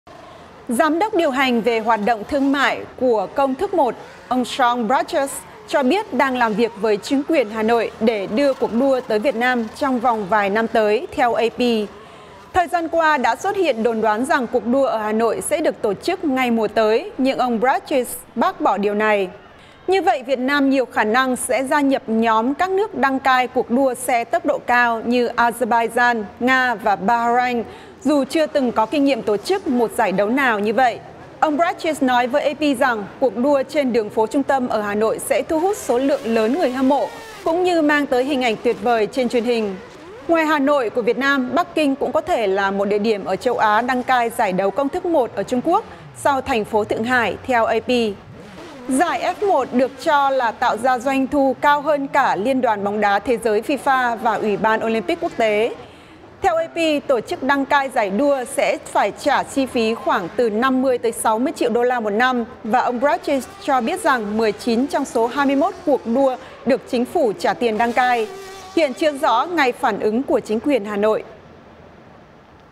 Hà Nội 'trong tầm ngắm' của cuộc đua Công thức Một
Tin tức Nếu không vào được VOA, xin các bạn hãy vào để vượt tường lửa. Giám đốc Điều hành về hoạt động thương mại của Công thức Một, Sean Bratches, cho biết đang “làm việc với chính quyền Hà Nội” để đưa cuộc đua tới Việt Nam “trong vòng vài năm tới”, theo AP.

Thời gian qua đã xuất hiện đồn đoán rằng cuộc đua ở Hà Nội sẽ được tổ chức ngay mùa tới, nhưng ông Bratches bác bỏ điều này.

Như vậy, Việt Nam nhiều khả năng sẽ gia nhập nhóm các nước đăng cai cuộc đua xe tốc độ cao như Azerbaijan, Nga và Bahrain, dù chưa từng có kinh nghiệm tổ chức một giải đấu nào như vậy.

Ông Bratches nói với AP rằng “cuộc đua trên đường phố trung tâm ở Hà Nội sẽ thu hút số lượng lớn người hâm mộ" cũng như mang tới “hình ảnh tuyệt vời trên truyền hình”.

Ngoài Hà Nội của Việt Nam, Bắc Kinh cũng có thể là một địa điểm ở châu Á đăng cai giải đấu Công thức Một ở Trung Quốc, sau thành phố Thượng Hải, theo AP.

Giải F1 được cho là tạo ra doanh thu cao hơn cả Liên đoàn Bóng đá Thế giới, FIFA, và Ủy ban Olympic Quốc tế.

Theo AP, tổ chức đăng cai giải đua sẽ phải trả phí khoảng từ 50 tới 60 triệu đôla một năm, và ông Bratches cho biết rằng 19 trong số 21 cuộc đua được chính phủ trả tiền đăng cai.

Hiện chưa rõ ngay phản ứng của chính quyền Hà Nội.